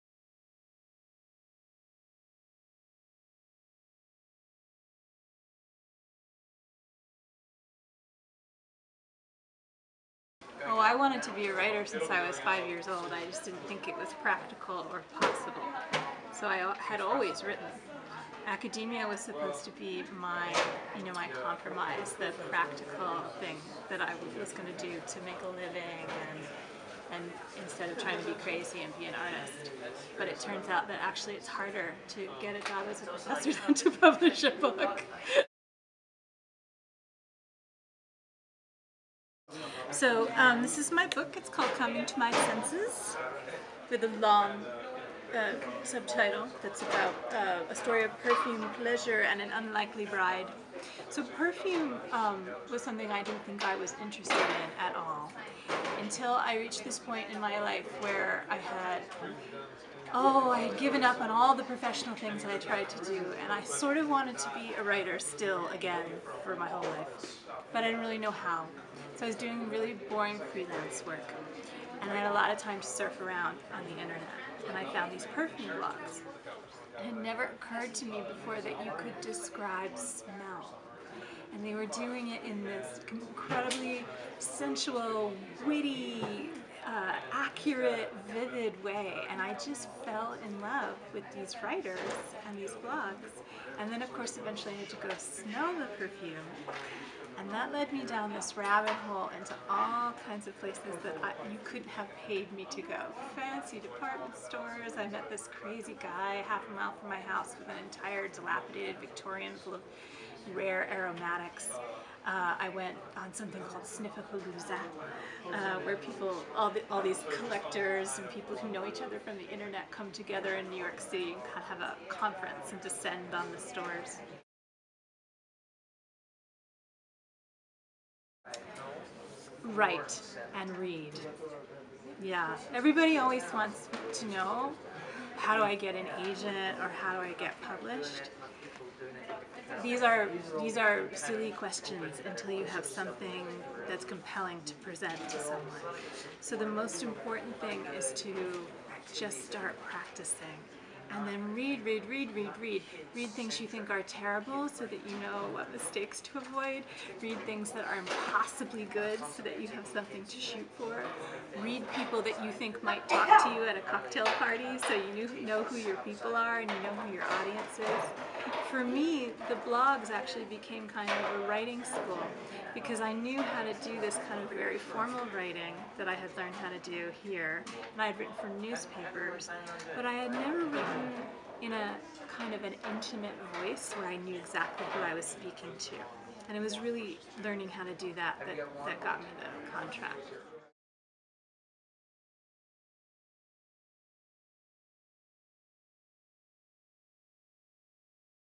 Undergraduate Writing Center Spotlight: Author Alyssa Harad
Alyssa Harad (Ph.D. English, '03) is the author of  "Coming to My Senses: A Story of Perfume, Pleasure, and an Unlikely Bride." Her writing has appeared in O, The Oprah Magazine, Marie Claire and the perfume blogs Now Smell This and Perfume-Smellin' Things.